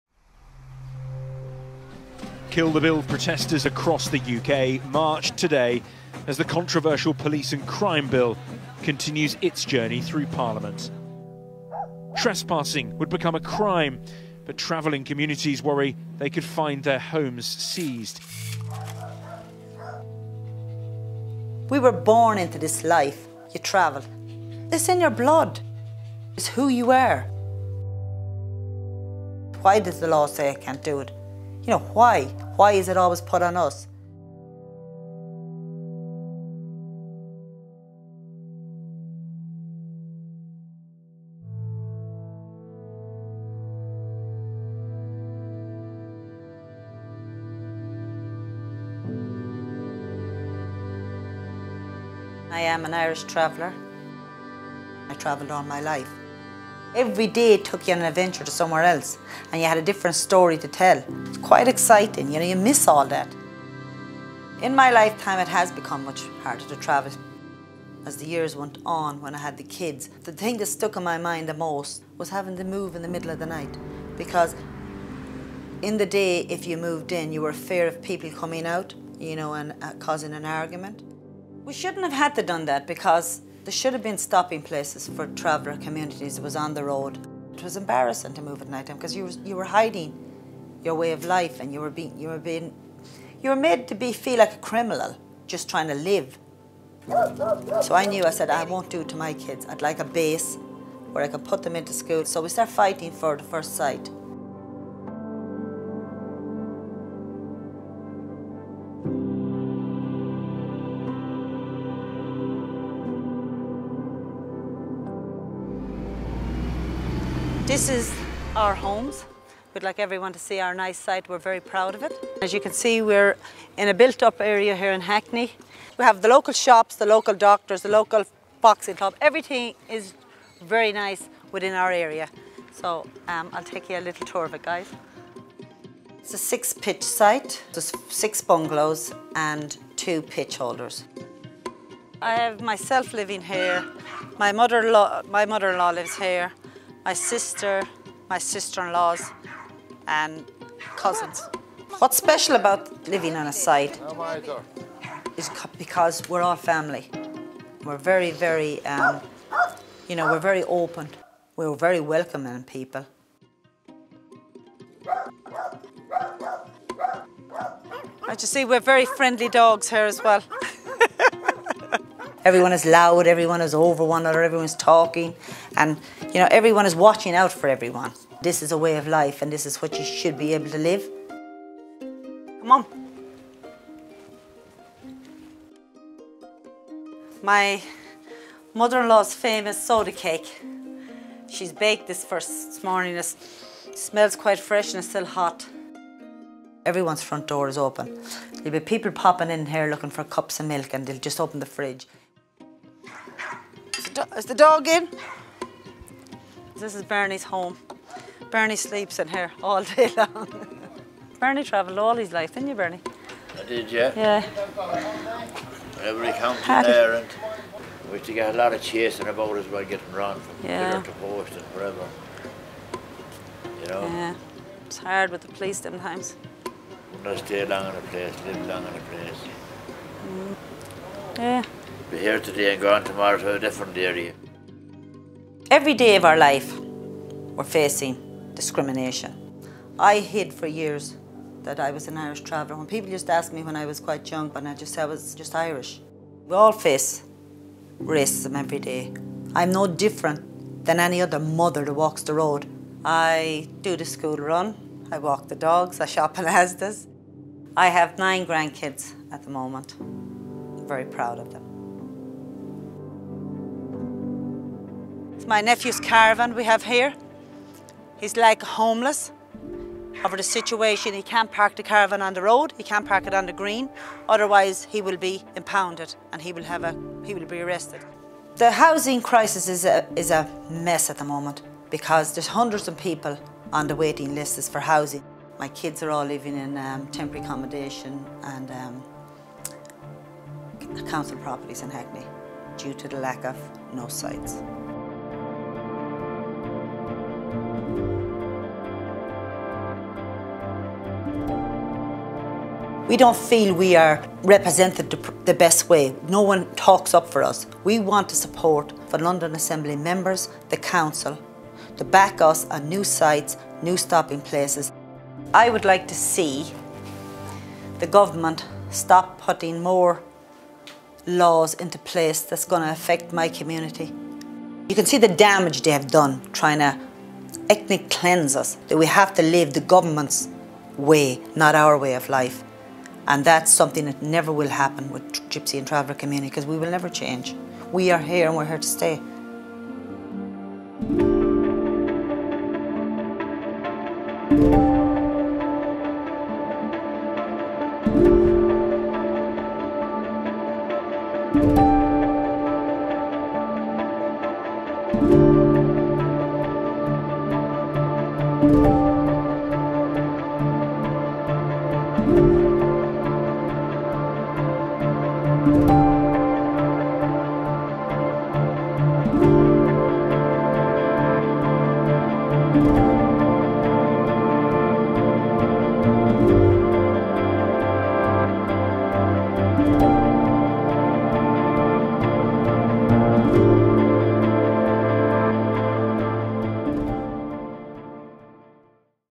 Nowhere left to turn - A film about the erosion of nomadism in the UK
Gypsies and Travellers have lived a nomadic way of life for centuries. But this way of life has been eroded over recent decades by a series of draconian laws.

For many years, local authorities have failed to fulfil their legal responsibilities to provide authorised family pitches on sites for Gypsies and Travellers – places where families can feel safe and get on with the business of living rather than being constantly moved on.

Now the government’s new Police Act has criminalised stopping on any public and private land. It is the last straw. Travellers who live a nomadic way of life have literally been left with nowhere to turn.